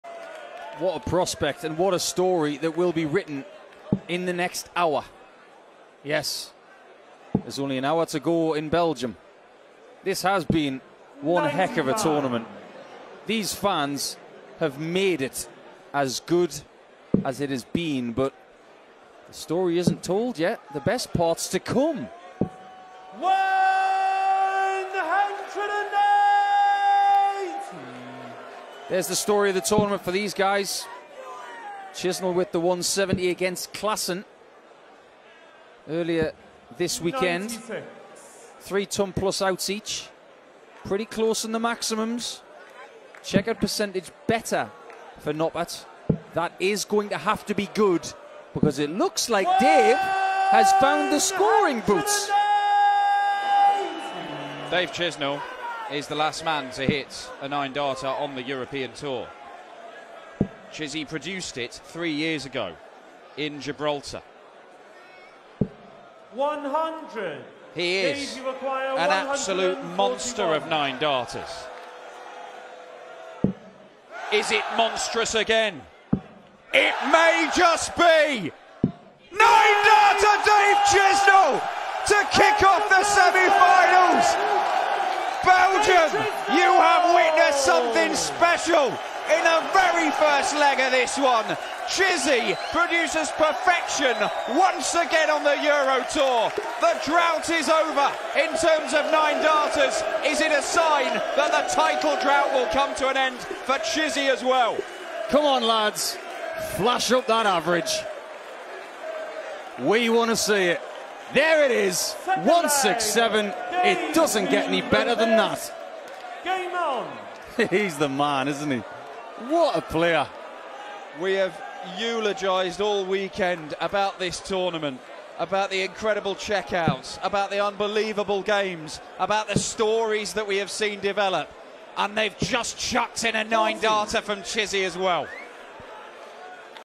NINE-DARTER! Dave Chisnall vs Danny Noppert | 2022 Belgian Darts Open
Absolutely incredible scenes as Dave Chisnall breaks the three-year wait for a Nine-Darter on the European Tour as he finds perfection in Wieze, Belgium.